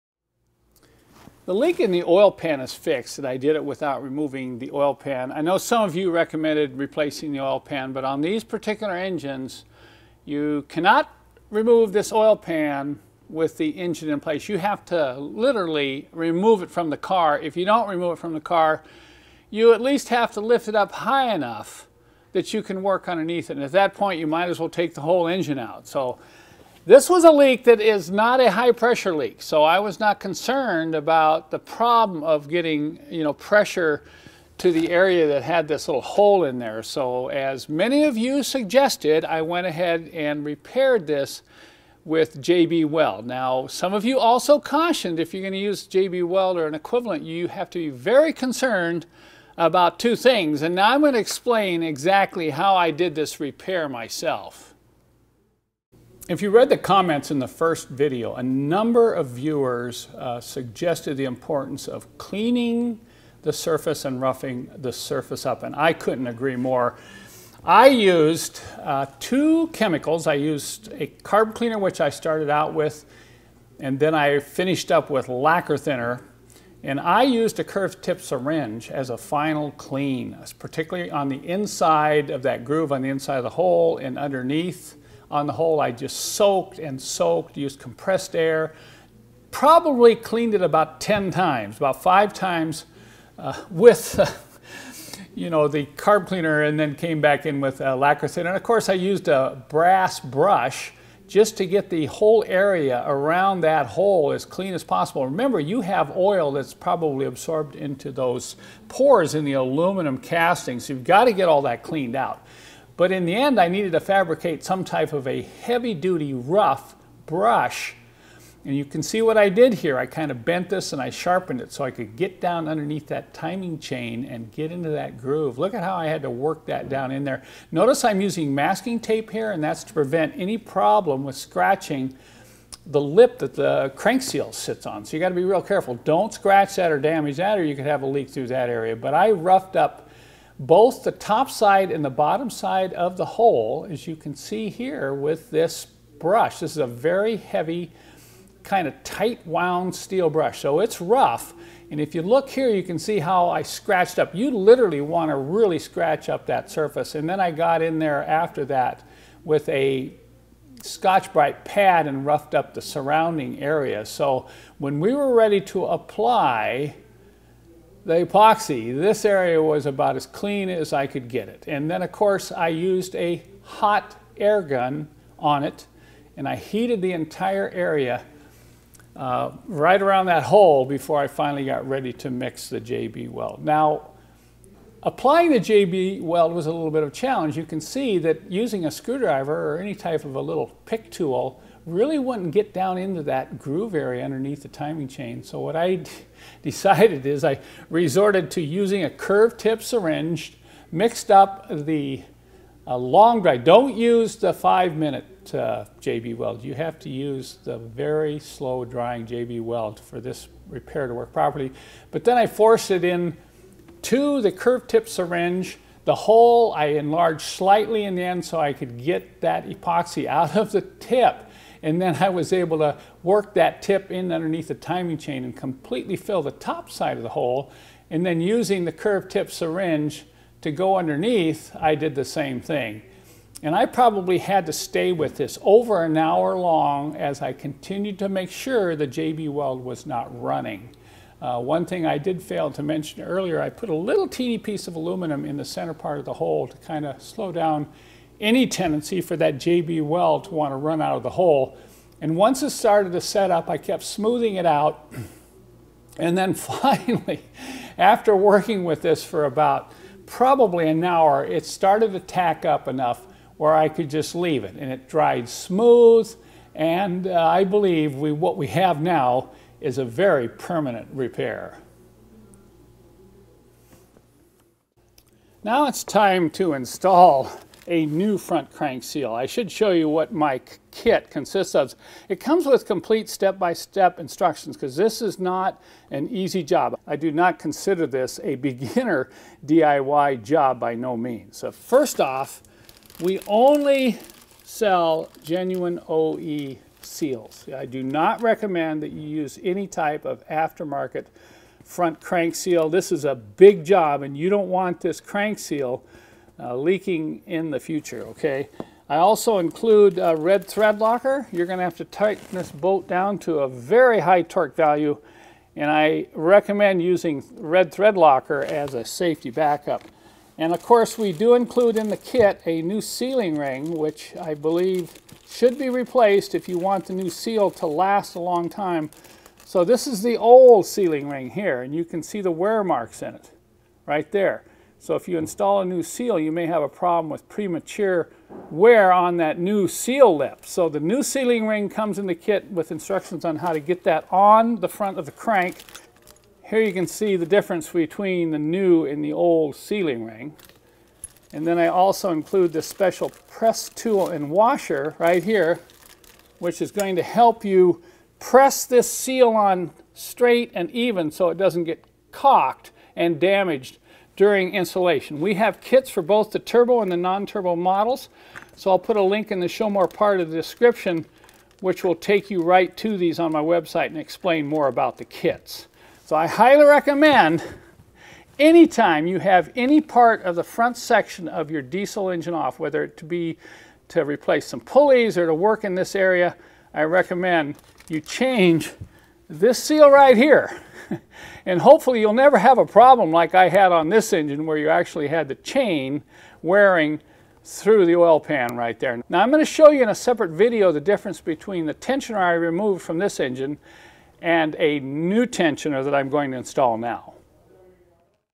How to Fix a Low Pressure Oil Leak Hole in an Aluminum Engine Oil Pan
Link to CRANK SEAL KITS Of course you could change the oil pan, but in this case the engine would have to be removed to get the pan off. To see what caused this oil leak see this video (PART 1) In this video PART 2 I will show you how I specifically fixed the oil leak hole in this pan without great expense or endless hours of work... link to video showing the failed tensioner